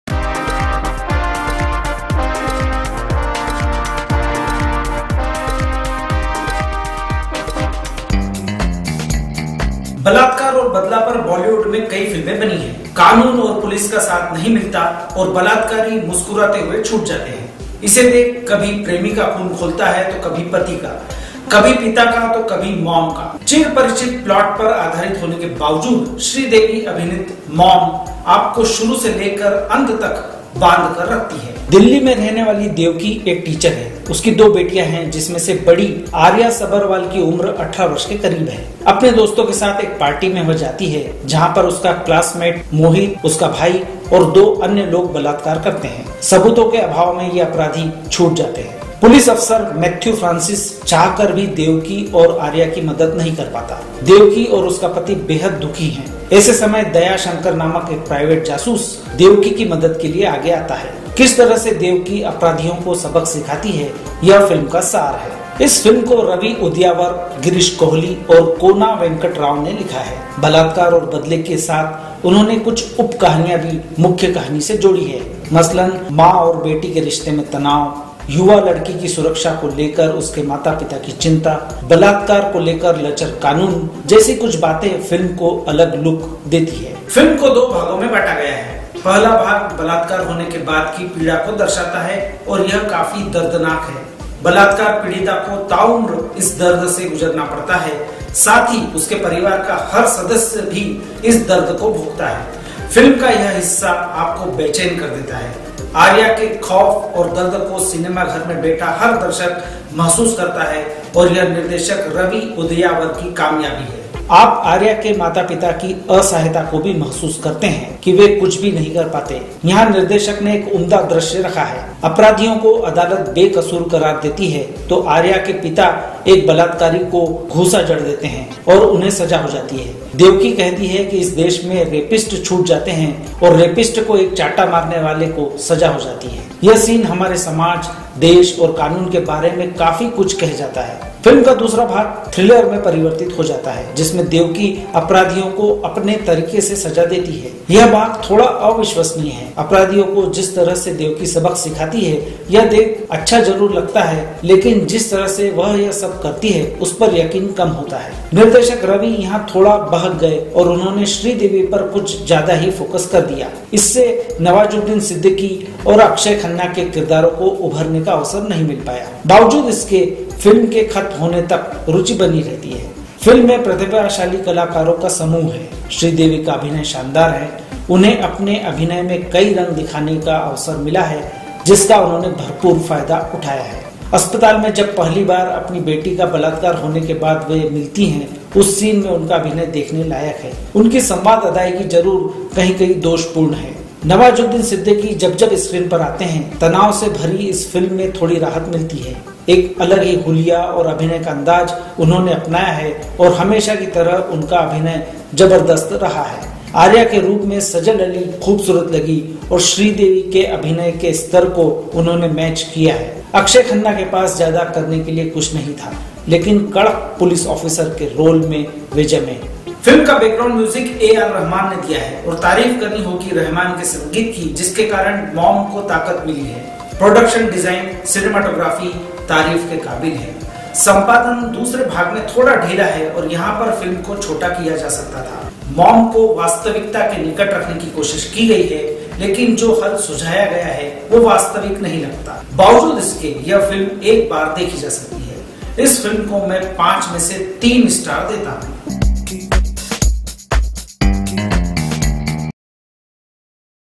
Mom: movie review, मॉम : फिल्म समीक्षा
बलात्कार और बदला पर बॉलीवुड में कई फिल्में बनी हैं। कानून और पुलिस का साथ नहीं मिलता और बलात्कारी मुस्कुराते हुए छूट जाते हैं। इसे देख कभी प्रेमी का खून खौलता है तो कभी पति का। कभी पिता का तो कभी 'मॉम' का। चिर-परिचित प्लॉट पर आधारित होने के बावजूद श्रीदेवी अभिनीत मॉम आपको शुरू से लेकर अंत तक बांध कर रखती है।